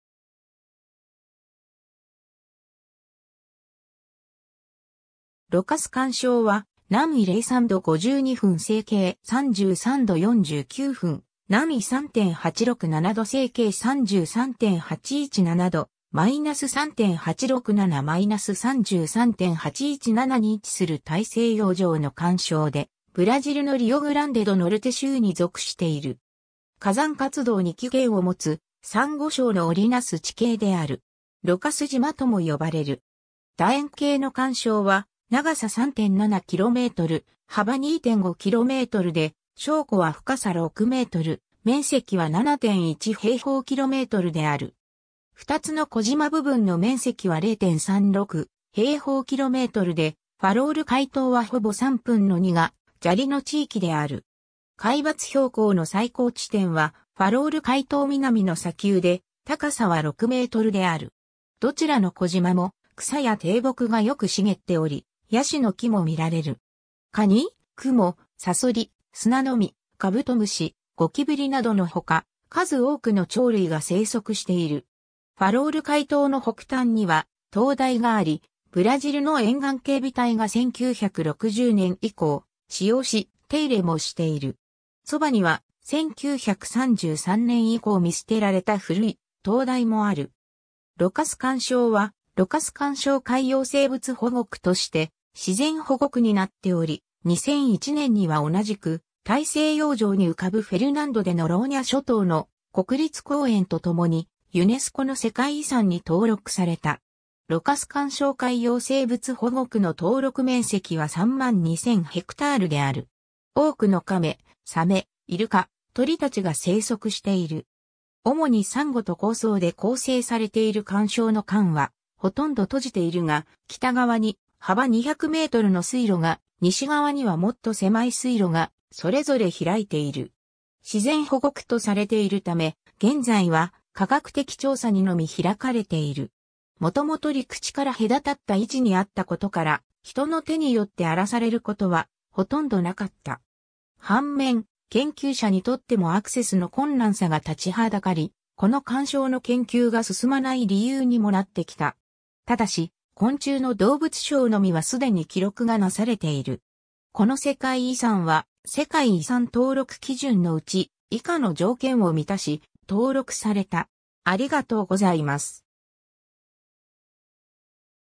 ロカス環礁
リオグランデ・ド・ノルテ州
ロカス環礁（Atol das Rocas）は、 南緯03度52分 西経33度49分 / 南緯3.867度 西経33.817度 / -3.867; -33.817に位置する大西洋上の環礁で、ブラジルのリオ・グランデ・ド・ノルテ州に属している。
火山活動に起源を持つ、サンゴ礁のおりなす地形である。
ロカス島とも呼ばれる。
 楕円形の環礁は長さ 3.7 km、幅 2.5 km で、礁湖は深さ 6m 、面積は 7.1 km2 である。
2つの小島部分（南西のセミテリオ Cemitério 島、北西のファロル・カイ Farol Cay 島）の面積は 0.36 km2 で、ファロル・カイ島はほぼ3分の2が砂利の地域である。
海抜標高の最高地点はファロル・カイ島南の砂丘で、高さは6 mである。
どちらの小島も草や低木がよく茂っており、ヤシの木も見られる。
カニ、クモ、サソリ、スナノミ、カブトムシ、ゴキブリなどのほか、数多くの鳥類が生息している。
 ファロル・カイ島の北端には灯台があり、ブラジルの沿岸警備隊が1960年以降使用し、手入れもしている。
そばには、1933年以降見捨てられた古い灯台もある。
 ロカス環礁は「ロカス環礁海洋生物保護区」（Reserva Biológica Marinha do Atol das Rocas, Biological Marine Reserve of Rocas Atoll）として自然保護区になっており、2001年には同じく大西洋上に浮かぶフェルナンド・デ・ノローニャ諸島の国立公園（フェルナンド・デ・ノローニャ国立海洋公園）とともに、ユネスコの世界遺産に登録された。
ロカス環礁海洋生物保護区の登録面積は 32000 ha である。
 多くのカメ、サメ、イルカ、鳥たちが棲息している。
主にサンゴと紅藻で構成されている環礁の環はほとんど閉じているが、北側に幅 200m の水路が、西側にはもっと狭い水路が、それぞれ開いている。
 自然保護区とされているため、現在は科学的調査にのみ開かれている。
もともと陸地から隔たった位置にあったことから、人の手によって荒らされることはほとんどなかった。
反面、研究者にとってもアクセスの困難さが立ちはだかり、この環礁の研究が進まない理由にもなってきた。
ただし、昆虫の動物相のみは既に記録がなされている。